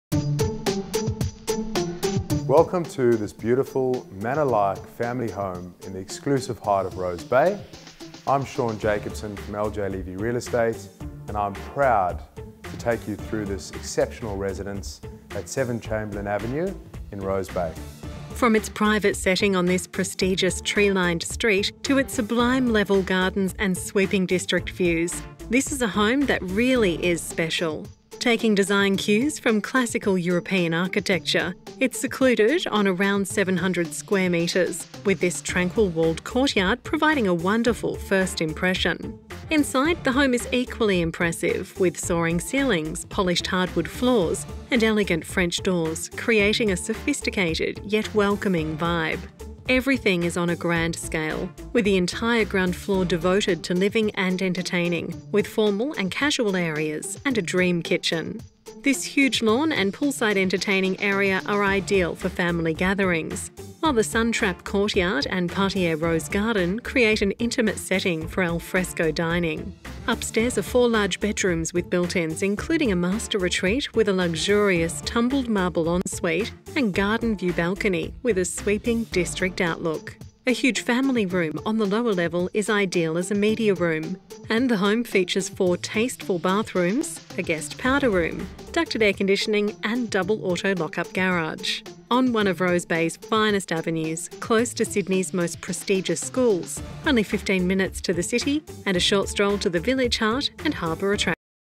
7 Chamberlain Avenue, Rose Bay - LJ Levi
Call Sean Jacobson - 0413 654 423

Inspired by classical European architecture, this manor-like family home rests amid established level gardens on a private approx 700sqm block. Grandly scaled interiors with fluid living areas showcase a refined contemporary aesthetic with all rooms opening outdoors via French doors. A huge lawn and poolside entertaining area are ideal for family gatherings while a suntrap walled courtyard and parterre rose garden create an intimate setting for entertaining. On one of Rose Bay's finest avenues, it's a short stroll to the chic village heart and harbour attractions.

- Sublime garden setting, elegant double-height entry hall
- 4 large beds with built-ins, 2 ensuite, guest powder room
- Master retreat with balcony and sweeping district views
- Free-flowing formal and casual living and dining areas
- Designer entertainer's CaesarStone island gas kitchen
- Huge family living or media room, r/c air conditioning
- 4 stylish baths, large laundry, custom cabinetry, storage
- Polished hardwood floors, Juliet balconies, double garage